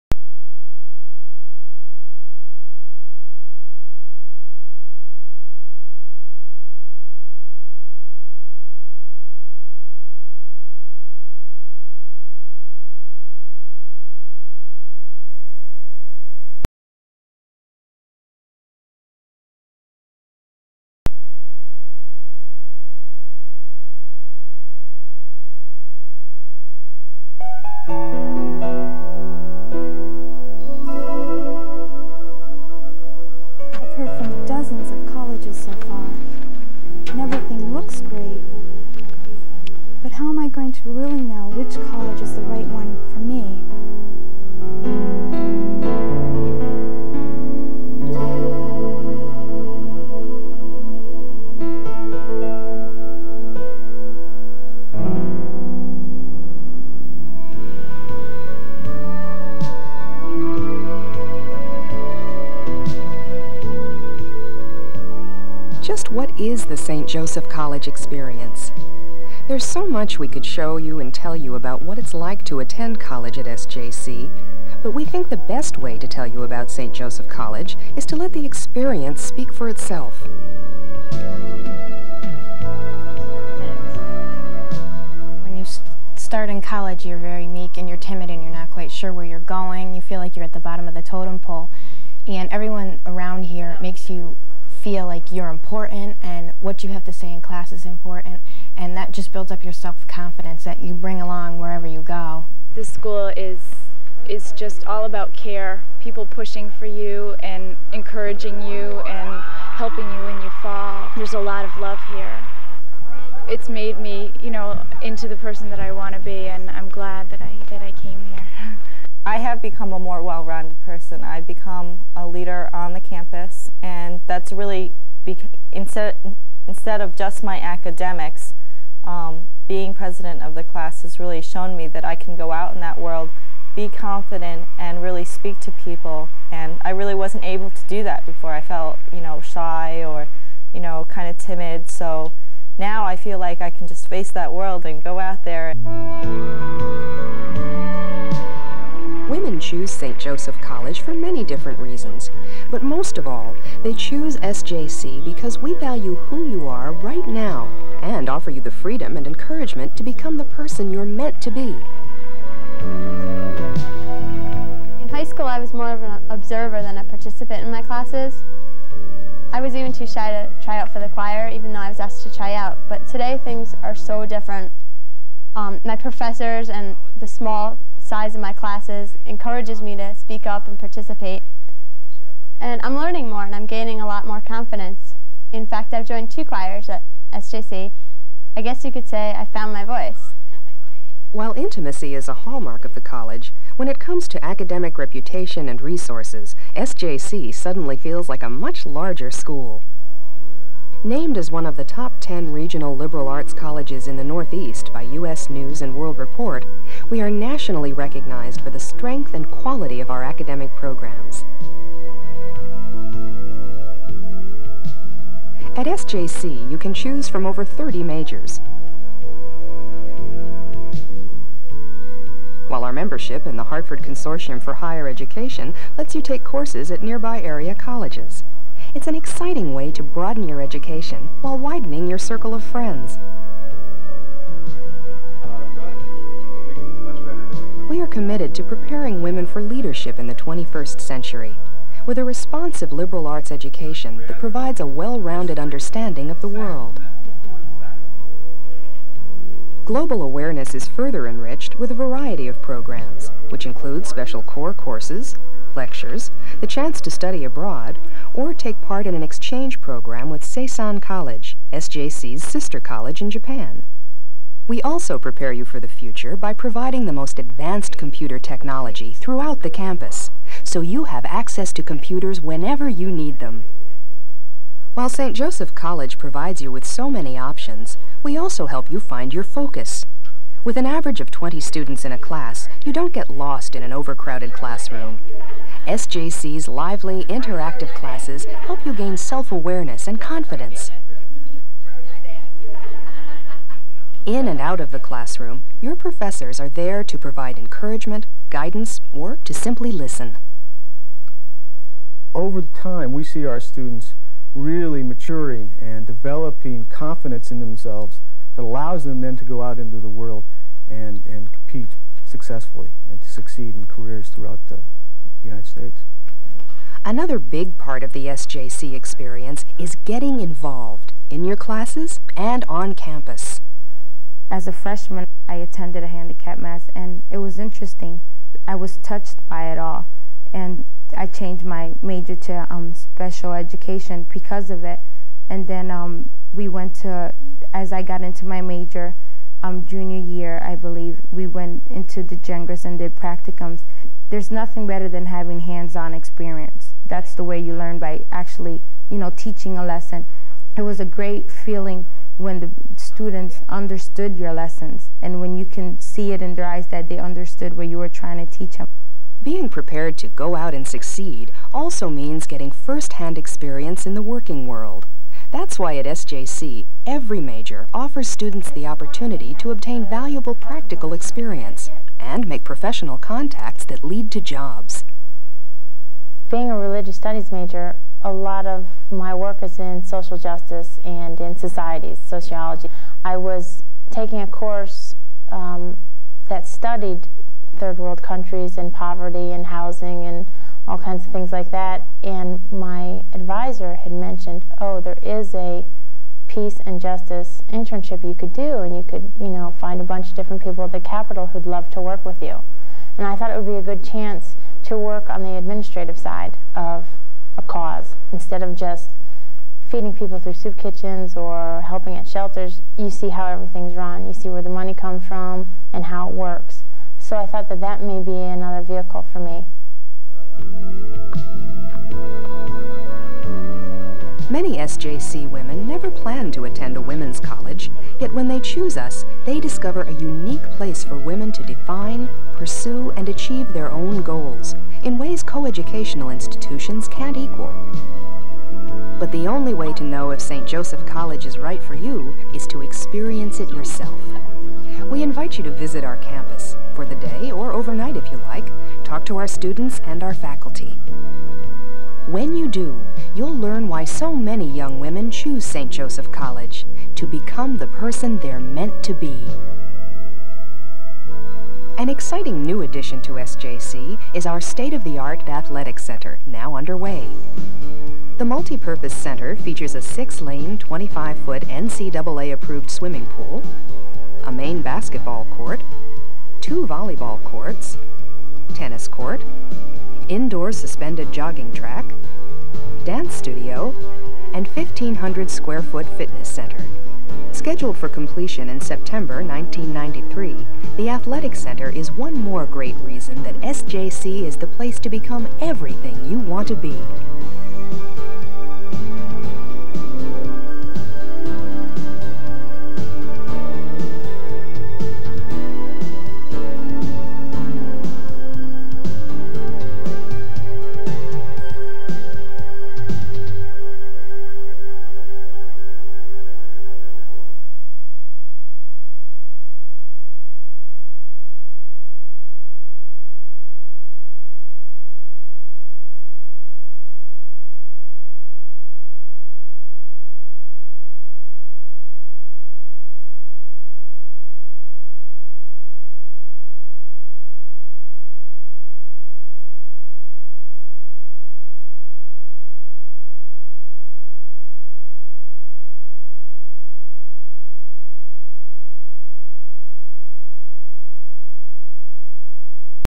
1990's Saint Joseph College Admissions Video
An admissions video from Saint Joseph College in Connecticut. Likely dates to the 1990's.